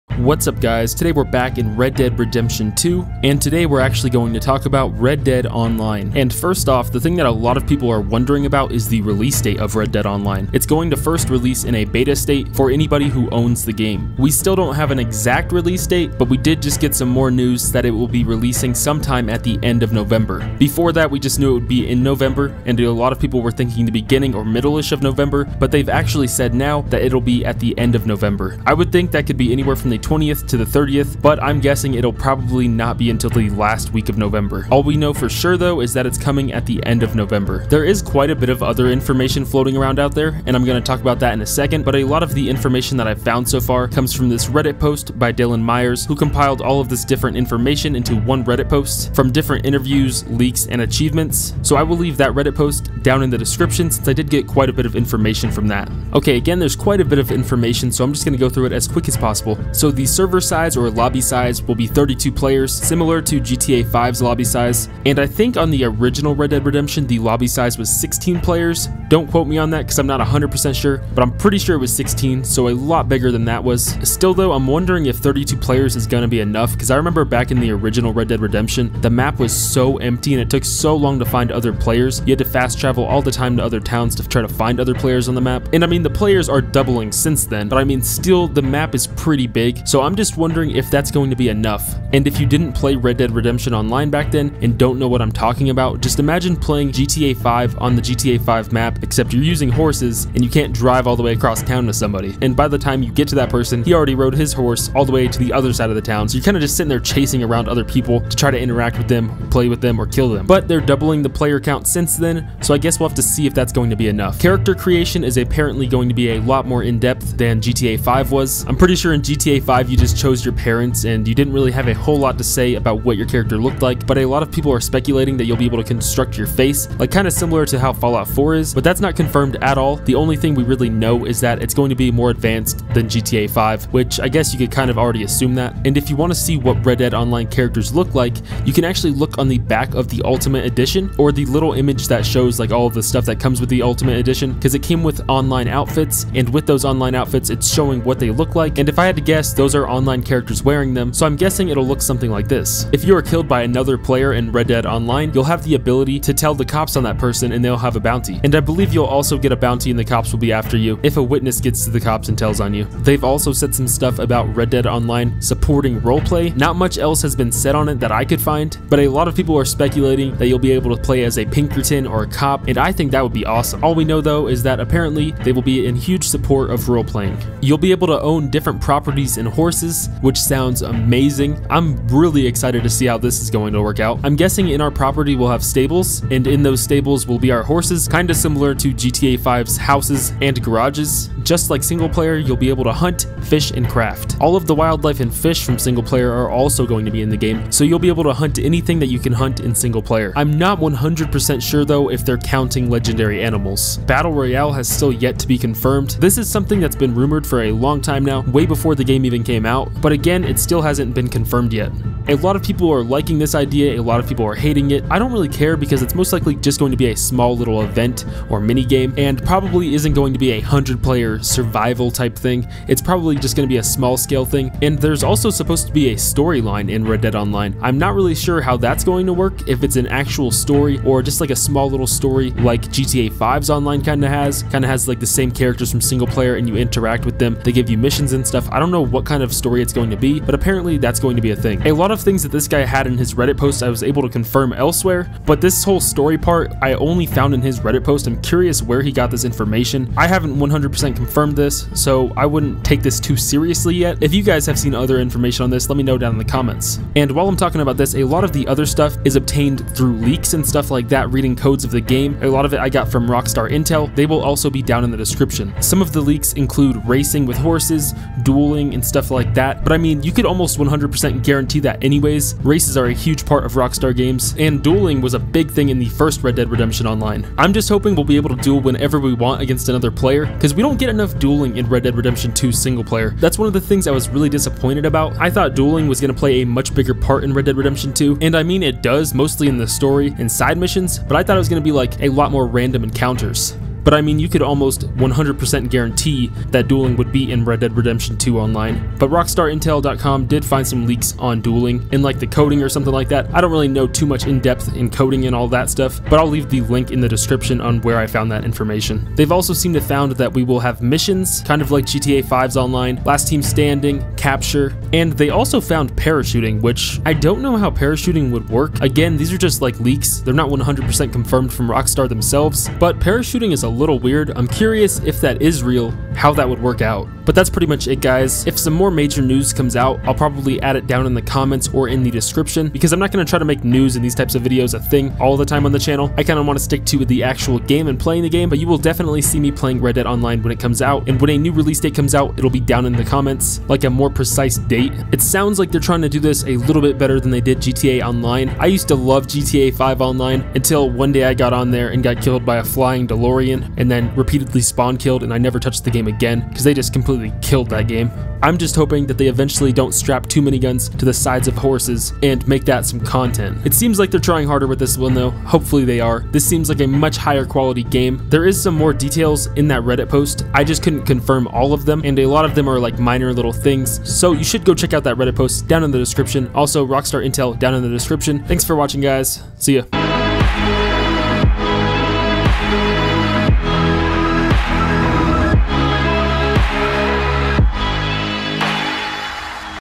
Red Dead Redemption 2 Online News & What We Know! Multiplayer, Release Date & Character Creation
I'll have a new RDR2 single player video up tomorrow. These won't be a usual thing. I tried smashing in as much info as possible and as quick as possible but below are some sources I used and you can find additional news and info there!

Sources:
Rockstar Intel
Duels, Parachuting, Racing, Last Team Standing & Leaks

Reddit post compiling a lot of Red Dead Online Info